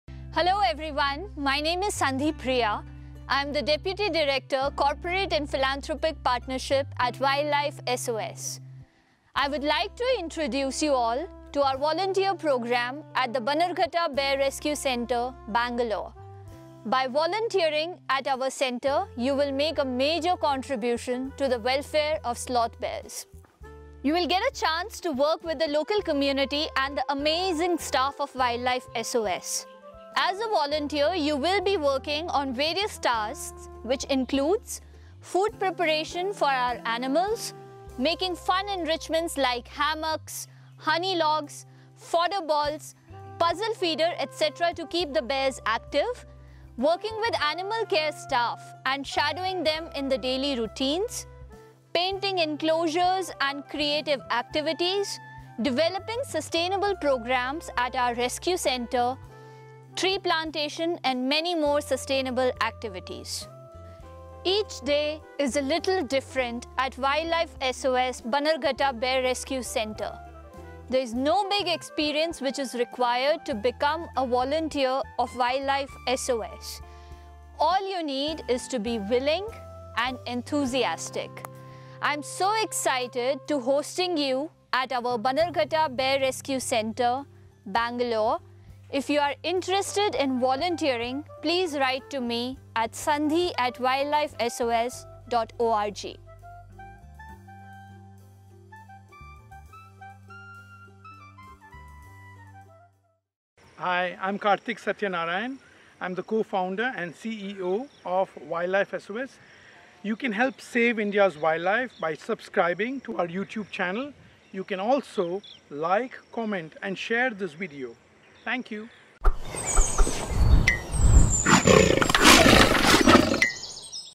Make A Difference For The Voiceless On This Volunteer Recognition Day!
Volunteers have always been the backbone of an organisation, and we at Wildlife SOS are here to offer you a unique opportunity! Join us at the Bannerghatta Bear Rescue Centre in Bengaluru, which provides long term care to over 60 rescued Sloth bears.

As a volunteer, you will get a chance to build hammocks, honey logs and fodder balls for these bears. Help keep our animals active by knitting coil ropes to hide enrichments. Other fun activities include gardening, distribution of food, routine observation and so much more.

Engage your creative spirit while working towards animal welfare today.